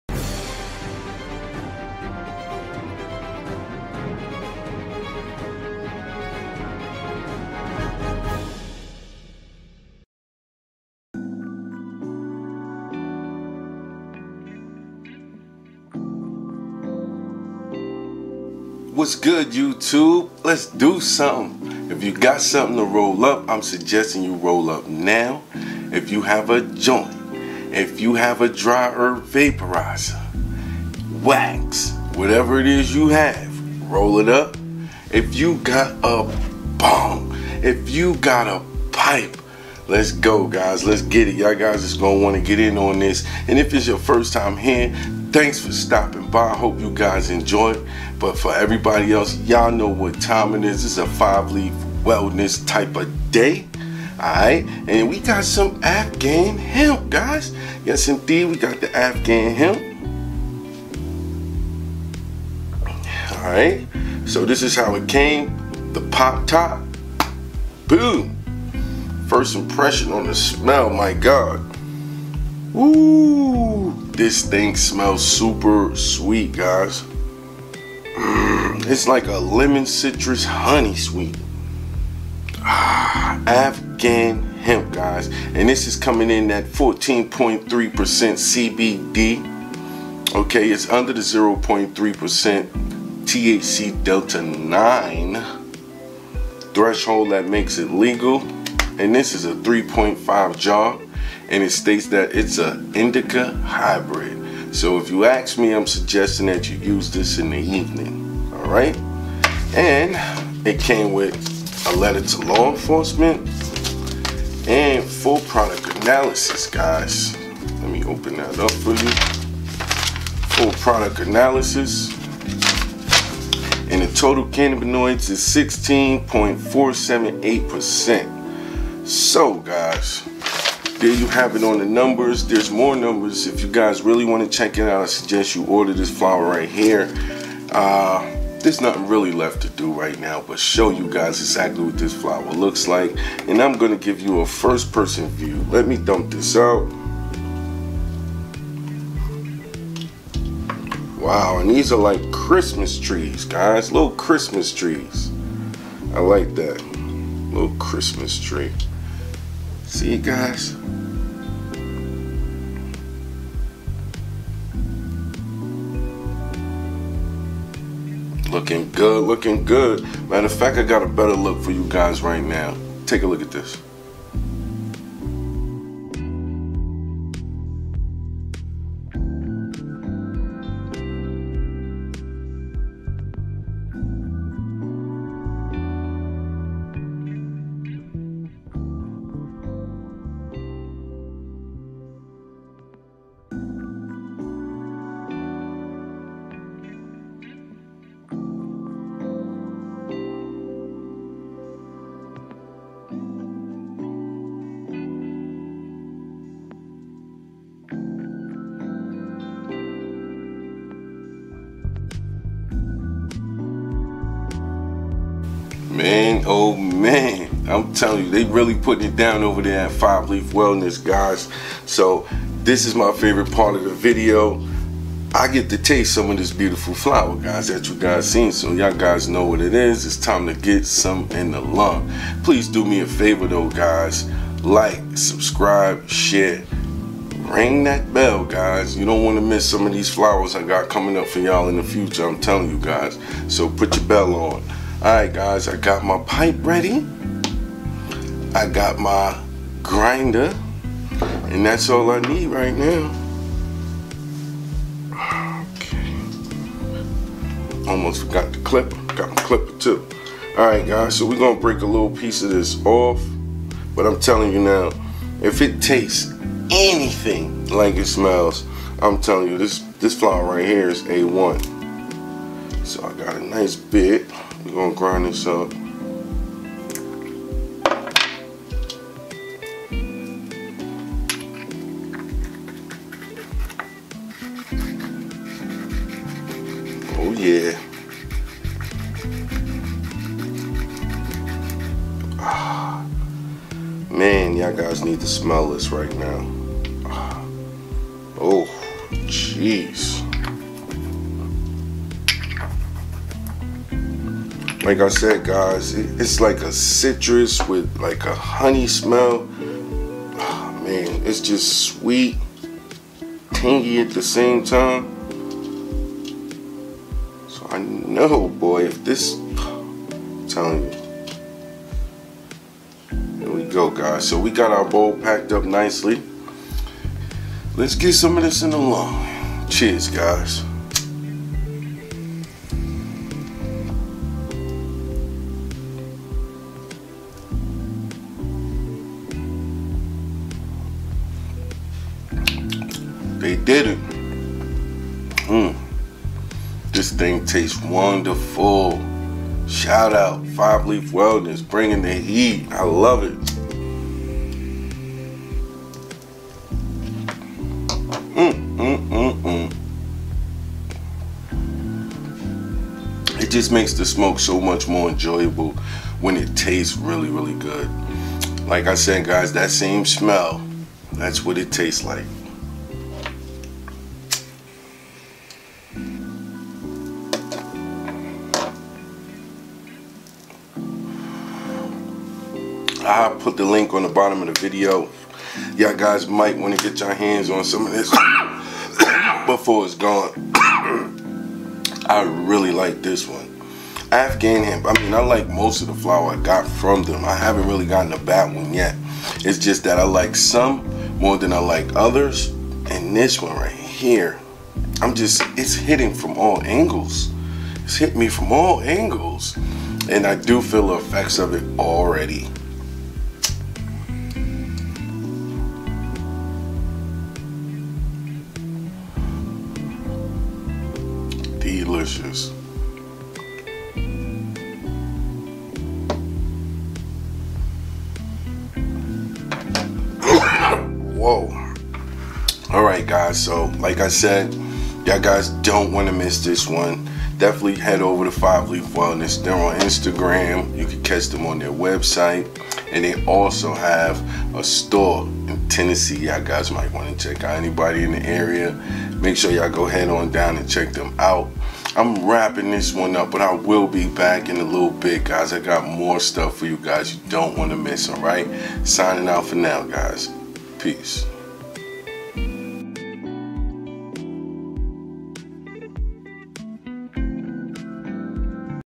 ( Afghan Hemp) from FIVE LEAF WELLNESS
[  Afghan hemp is an all around great flower ]

1.The smell is amazing
Super Gas ⛽ With a sweat floral aroma

2. The taste is a match to the smell

3. Its a clean smooth smoke, and also packes a lil punch.

4. The effect were calming and relaxing with a full body comfort

You can get some here👇
five leaf wellness .com

All products shown were federally legal due to the 2018 farm bill.